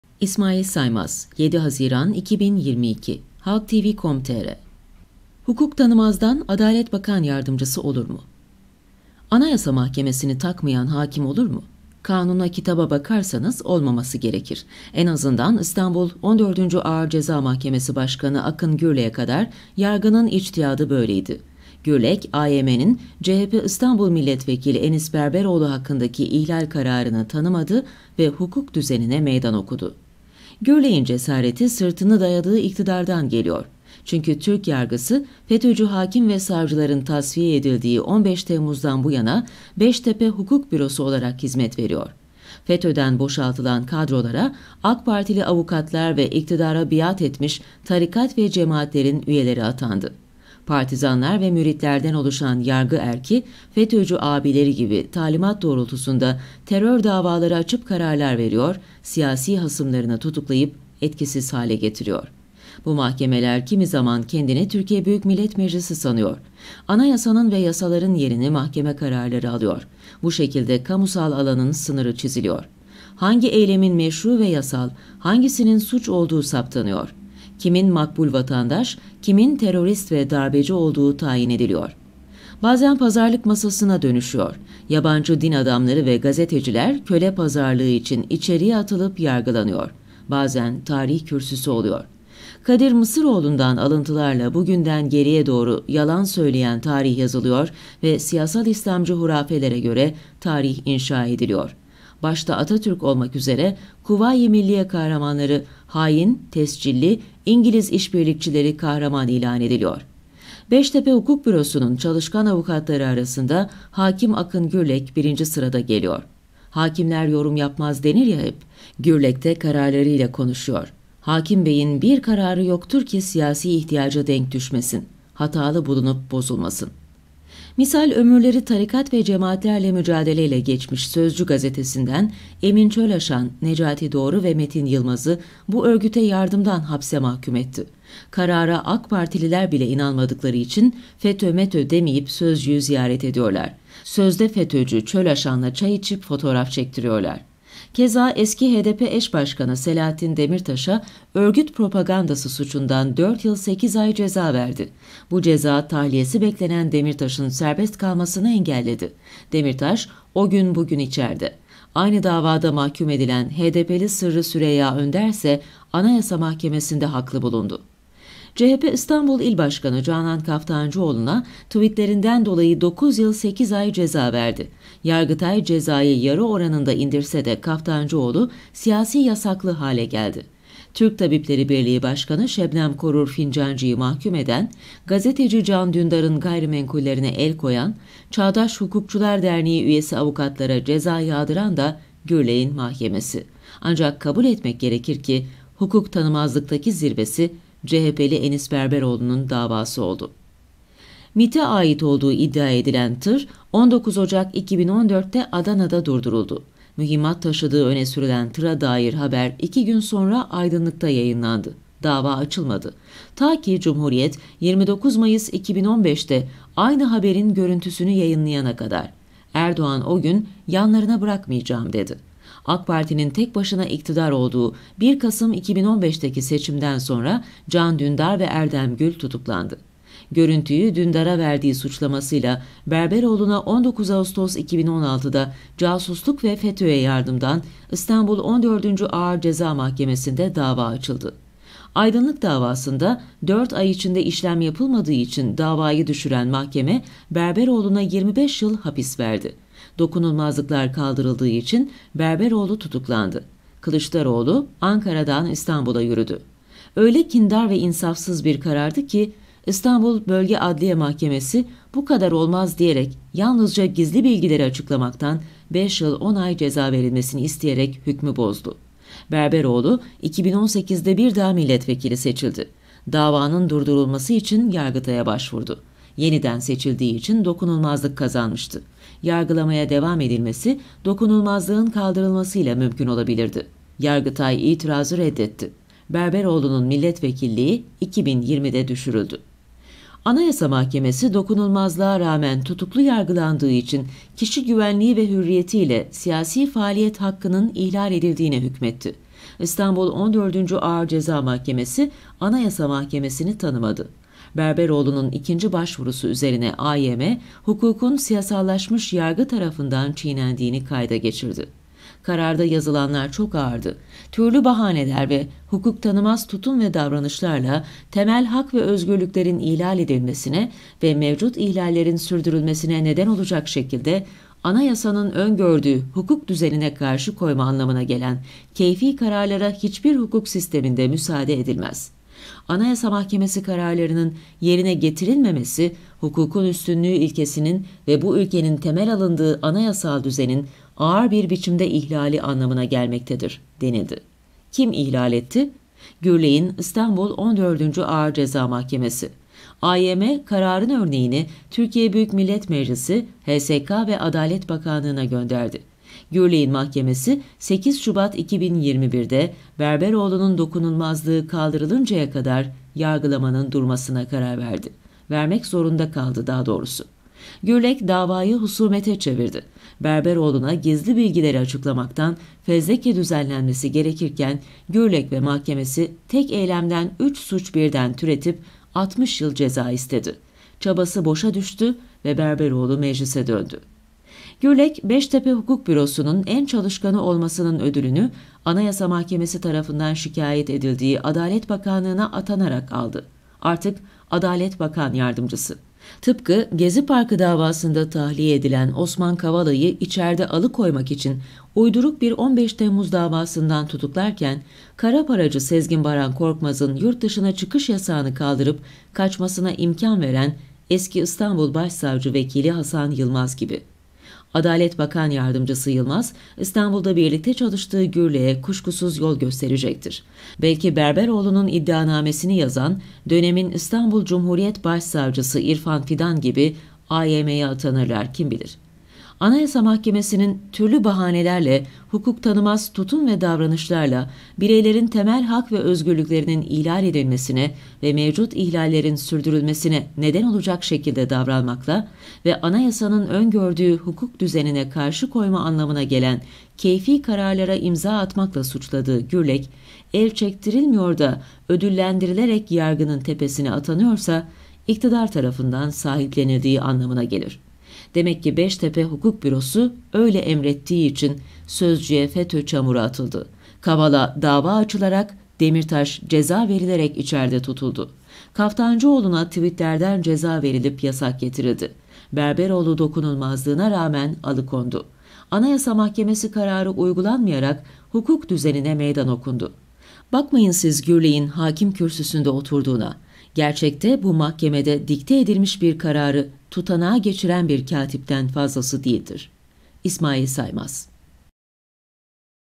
İSMAİL SAYMAZ - Hukuk Tanımazdan Adalet Bakan Yardımcısı Olur mu? * Sesli Köşe Yazısı Dinle *
Şu anda, ' İsmail Saymaz 'ın 7 Haziran 2022 tarihinde ' HalkTv.com.tr 'de yayınlanan ' köşe yazısı 'nı dinliyorsunuz.